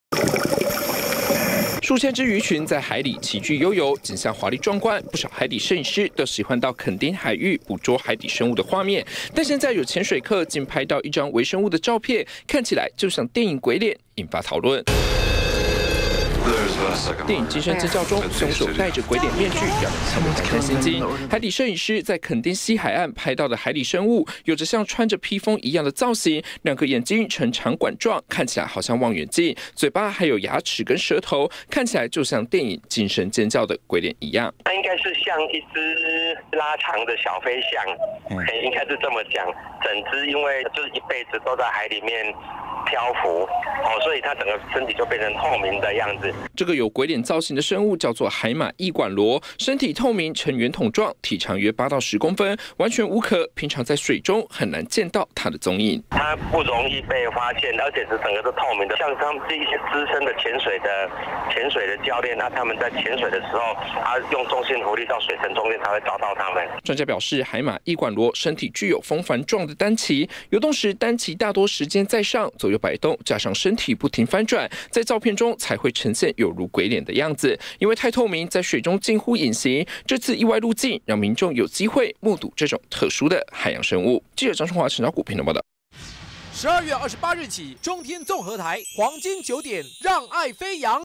20170110中天新聞 海底攝影見鬼臉？ 原來是「海馬翼管螺」
屏東一名資深海底攝影師，上個禮拜到墾丁西海岸潛水攝影，意外拍攝到一種名為「海馬翼管螺」的微小透明生物，放到電腦上檢視，看到照片中的海馬翼管螺，長相有如電影「驚笑尖叫」中的鬼臉，引發網友熱烈討論。